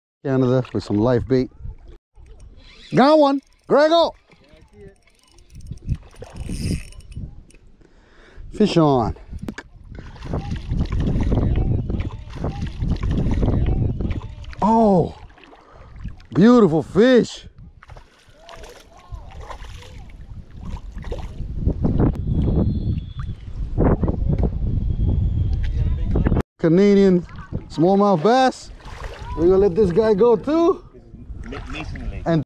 Smallmouth bass slams live minnow Canada 🇨🇦🎣 (Shimano Stradic- St Croix Triumph)
Fishing for Smallmouth bass in Ontario, Canada

Protect the environment. Don’t litter our fishing spots. Respect for all of us and nature.

Gear Used
-St Croix Triumph spinning rod
-Shimano Stradic Ci4+ 2500 reel
-Live bait (minnows)